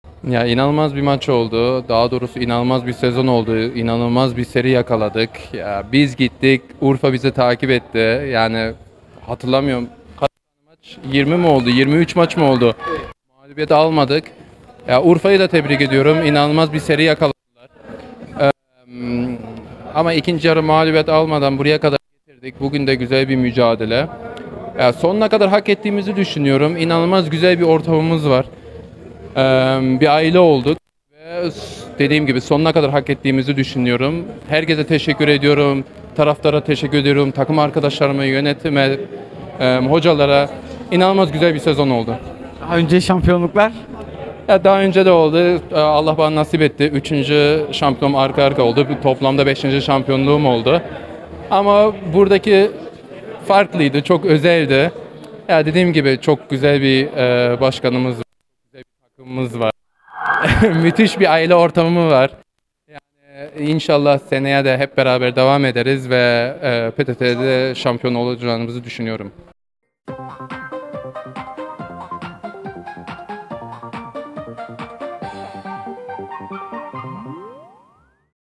Ferhat Yazgan: 1. Ligde de şampiyon olacağız!
Ferhat Yazgan şampiyonluk maçı sonrası seneye 1. Ligde de şampiyon olacağımızı düşündüğünü söyledi.